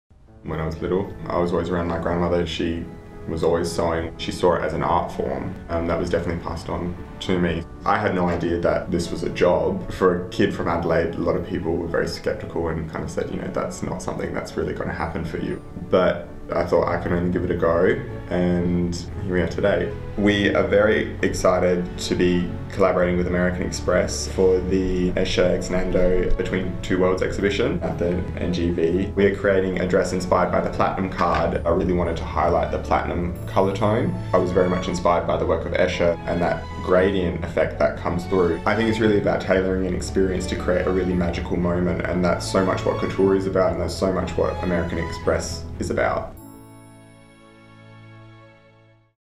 NGV Gala: Platinum Moment
We’re proud to be a major partner of the NGV Gala and back Australian designer, Paolo Sebastian, in creating a bespoke gown inspired by the American Express Platinum Card to celebrate the Escher X nendo | Between Two Worlds exhibition.